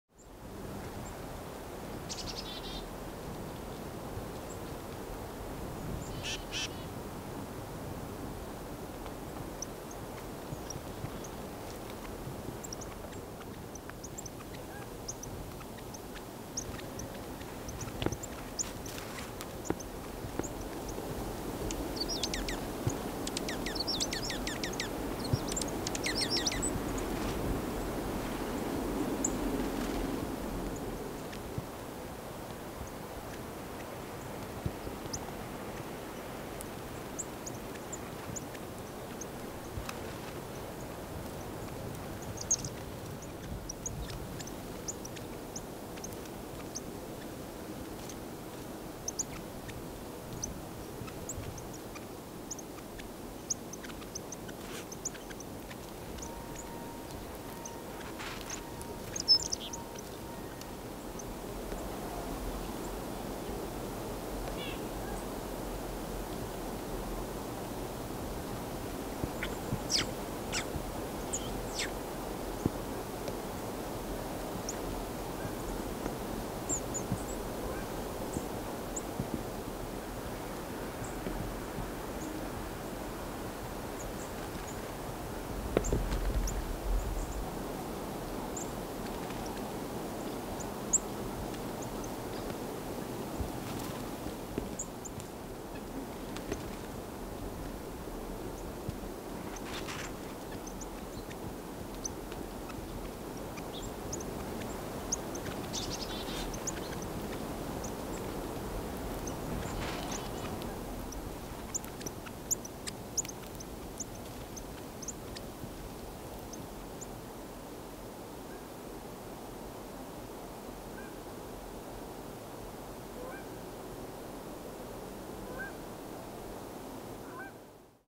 Nature: Songbirds in snow
We leave you this Sunday with songbirds in the snowy woods near Buffalo, N.Y. Videographer: Carl Mrozek.